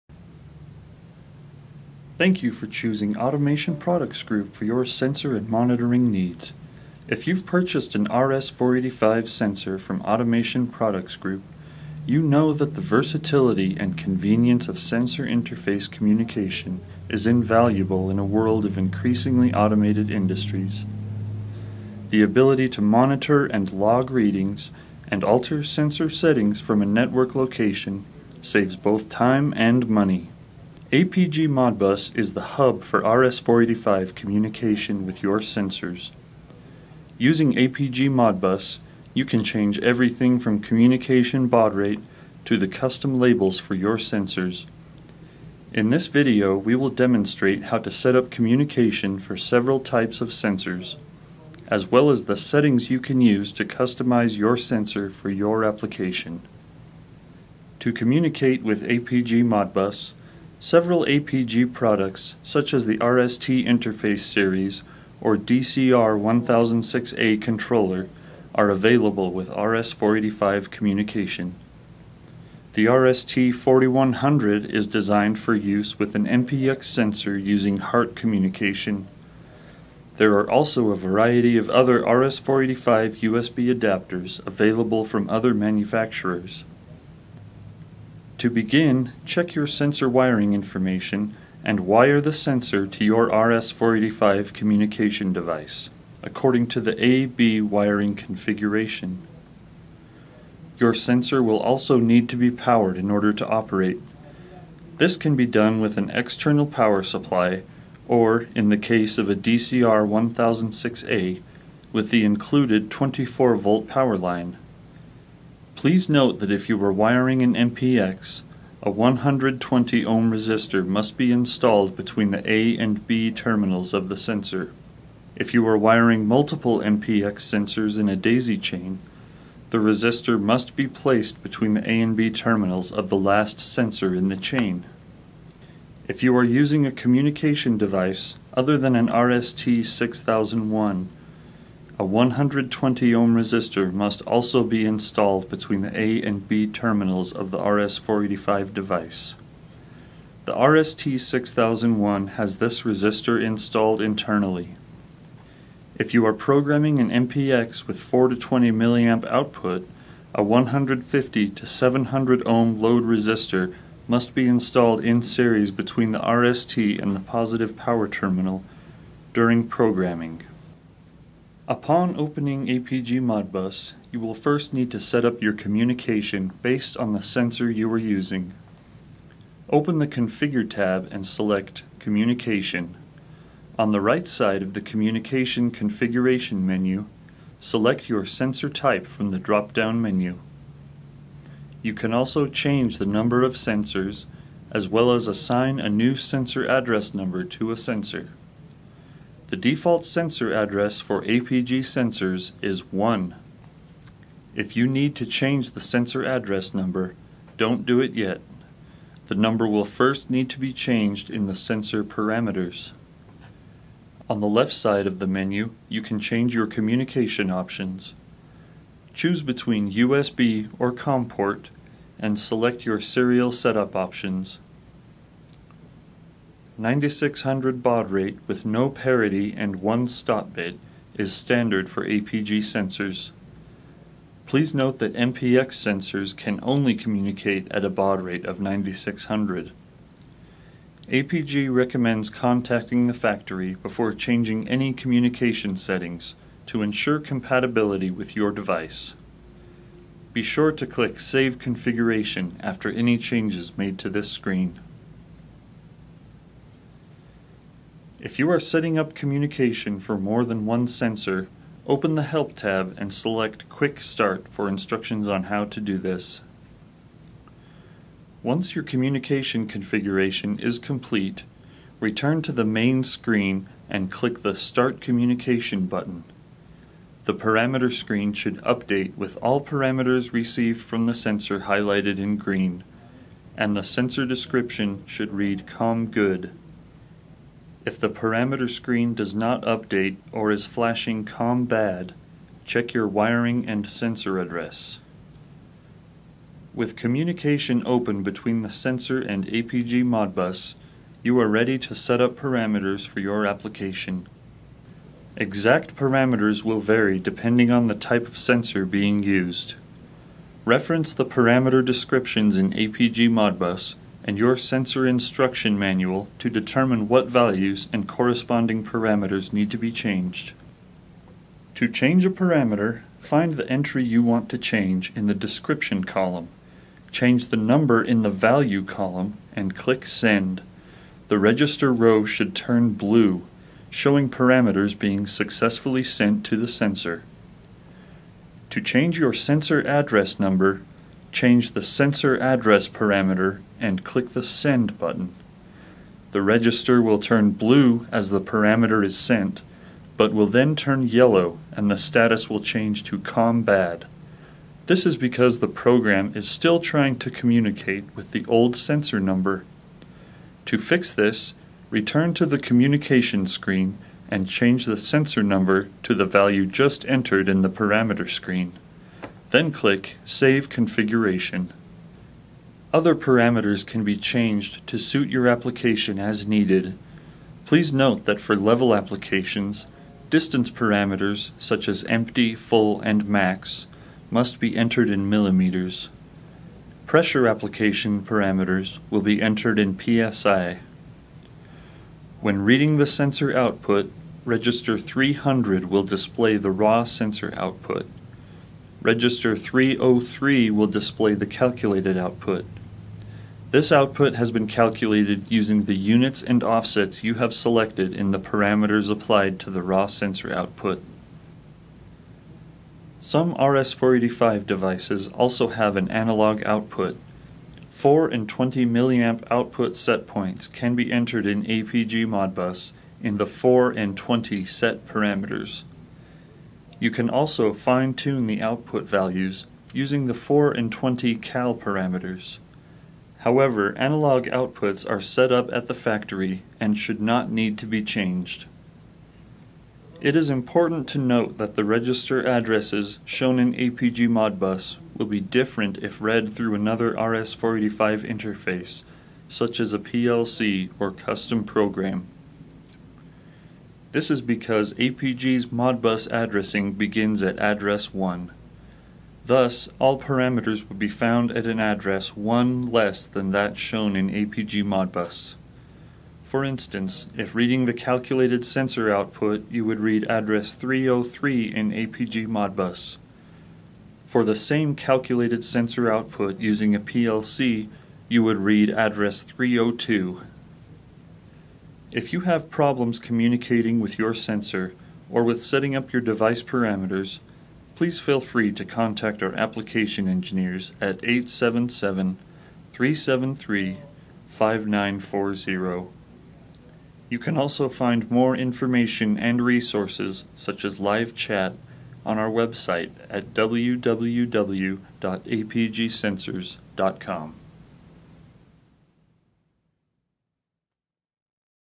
How To Setup Your Sensor with APG Modbus
In this video, we will demonstrate how to set up communication for several types of sensors, as well as the settings you can use to customize your sensor for your application.

00:22 APG MODBUS
Using APG Modbus, you can change everything from communication baud rate to the custom labels for your sensors.

01:28 CHECK YOUR SENSOR WIRING INFORMATION
Wire the sensor to your RS-485 communication device according to the A-B wiring configuration.

01:40 POWER YOUR SENSOR
Power your sensor with an external power supply. For the DCR-1006A, power with the included 24-volt power line.

01:55 MPX WIRING NOTE

02:51 SETTING COMMUNICATION
1. Open the Configure tab and select Communication.
2. On the right side of the Communication Configuration
menu, select your sensor type from the drop-down menu.

03:11 ASSIGNING NEW ADDRESS
You can also change the number of sensors, as well as assign a new address number to a sensor. The default sensor address for APG sensors is 1. If you need to change the sensor address number, don’t do it yet! The number will first need to be changed in the sensor parameters.

On the left side of the menu, you can change your communication options. Choose between USB or Comm port and select your serial setup options. 9600 baud rate with no parity and 1 stop bit is standard for APG sensors. Please note that MPX sensors can only communicate at a baud rate of 9600.

APG recommends contacting the factory before changing any communication settings to ensure compatibility with your device.

04:18 COMMUNICATION FOR MULTIPLE SENSORS
If you are setting up communication for more than one sensor, open the Help tab and select Quick Start for instructions on how to do
this.

04:56 SETTING PARAMETERS FOR YOUR APPLICATION
With communication open between the sensor and APG Modbus, you are ready to setup parameters for your application. Exact parameters will vary depending on the type of sensor being used. Reference the parameter descriptions in APG Modbus and your sensor instruction manual to determine what values and corresponding
parameters need to be changed.

1. To change a parameter, find the entry you want to change in the “Description” column.
2. Change the number in the “Value” column and click “Send”. The register row should blue, showing parameters being successfully sent to the sensor.
3. To change your sensor address number, change the Sensor Address parameter and click the “Send” button. The register will turn blue as the parameter is sent but will then turn yellow and the
status will change to Comm Bad. This is because the program is still trying to communicate with the old sensor number. To fix this, return to the Communication screen and change the sensor number to the value just entered in the parameter screen.
4. Then click “Save Configuration”.
5. Other parameters can be changed to suit your application as needed.

06:17 LEVEL APPLICATIONS
Level applications, distance parameters such as empty/full/max must be entered in millimeters.

06:27 PRESSURE APPLICATIONS
Pressure application parameters will be entered in PSI.

06:32 REGISTER 300 - RAW SENSOR OUTPUT
Register 300 will display the raw sensor output.

06:39 REGISTER 303 - CALCULATED OUTPUT
Register 303 will display the calculated output; this output has been calculated using the units and offsets you have selected in the
parameters applied to the raw sensor output.

6:53 ANALOG OUTPUT
Some RS-485 devices also have an analog output. You can change your analog output settings in APG modbus. 4 and 20 milliamp output setpoints can be entered in APG modbus in the 4 and 20 Set parameters.

08:02 NEED SUPPORT?
Engineers at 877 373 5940. You can also find more information and resources such as Live Chat on our

Follow us to get the most up-to-date APG news, blogs, videos, and white papers!